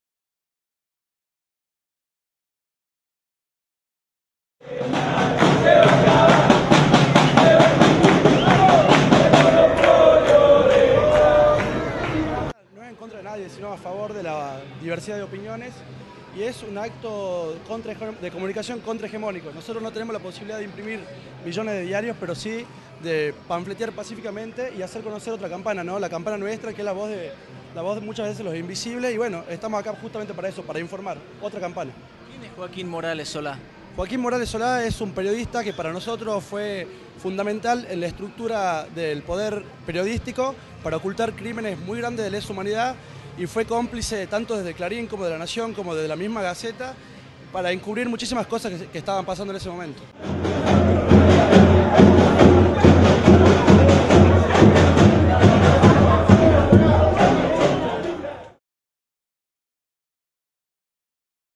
Protesta contra Morales Solá
Un grupo autodenominado Congreso Nacional de la militancia de Tucumán protestó frente al Virla contra la presencia del periodista Joaquín Morales Solá